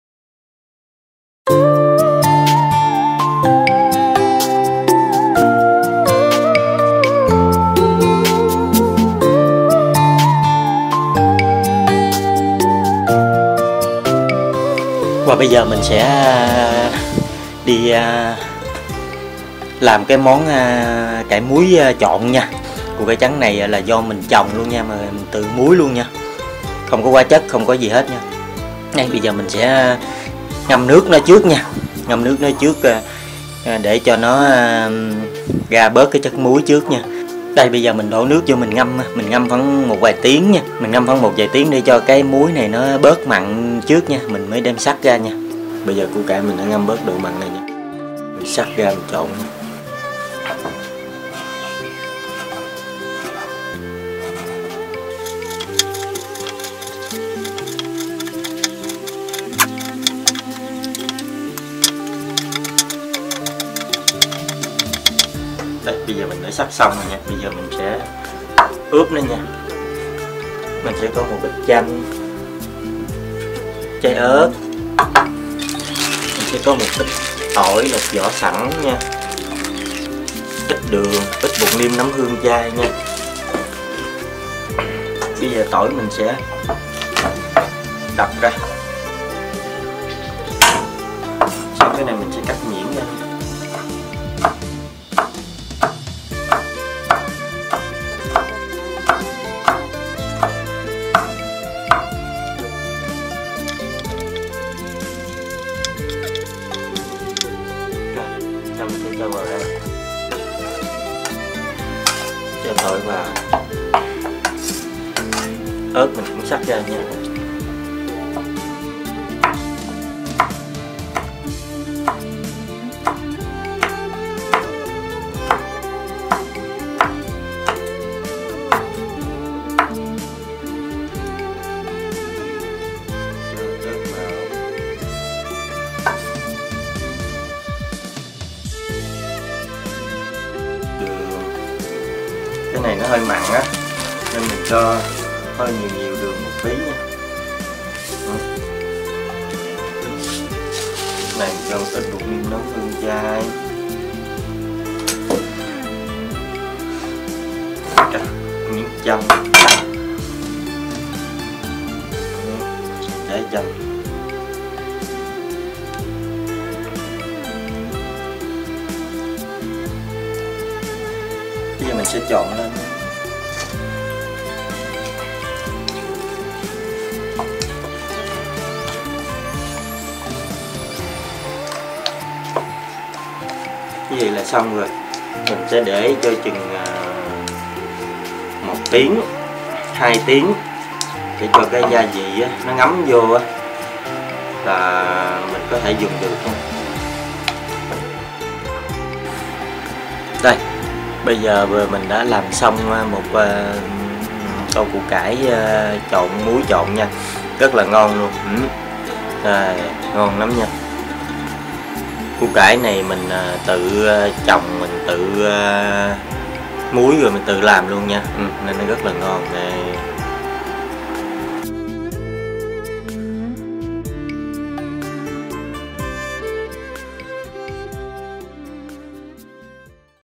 Củ Cải Muối Làm Cách này Rất Tốn Cơm, Ai Ở Miền Tây Chắc Sẽ Biết!!!!
củ cải muối trộn là món ăn rất được nhà nông ưa thích, bởi cái vị hơi mặn mặn,cay cay,chua chua và khi ăn rất giòn , thường được ăn với cơm nguội rất ngon luôn